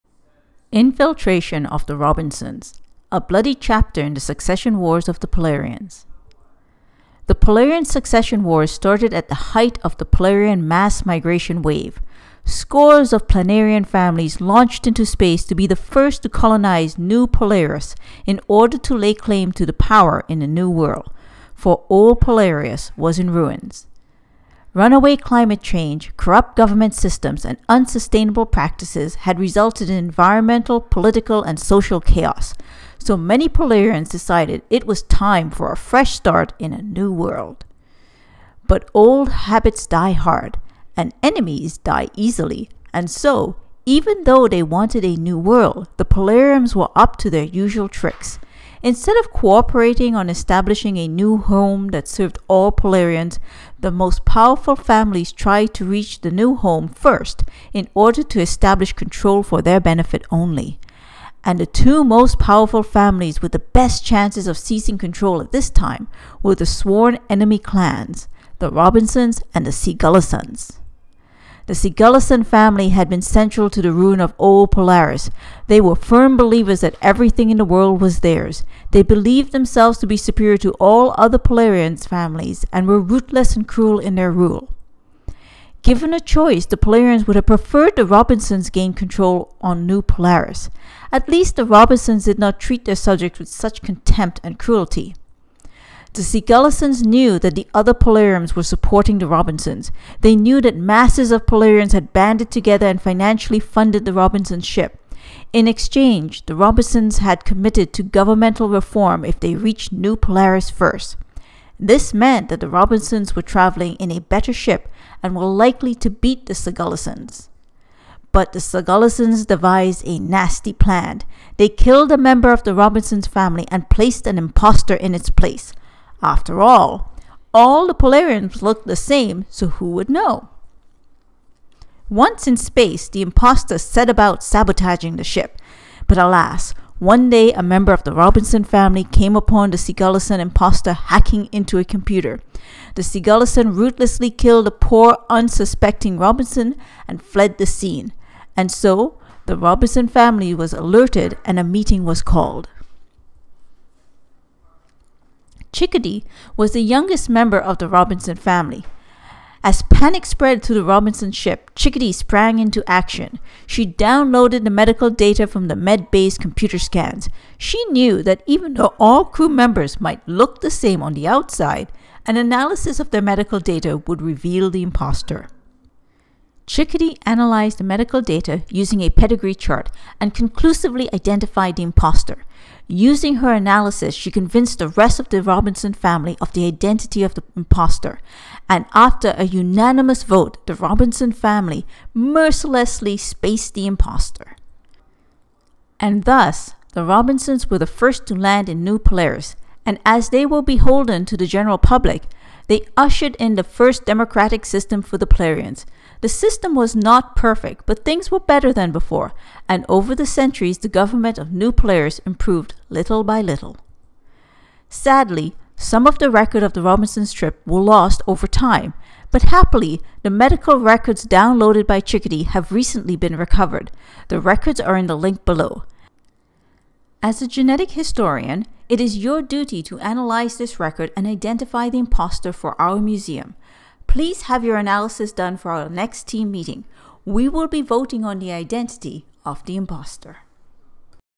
Robinson Murder Mystery
Grade 11 Biology Genetics Activity on Pedigree Charts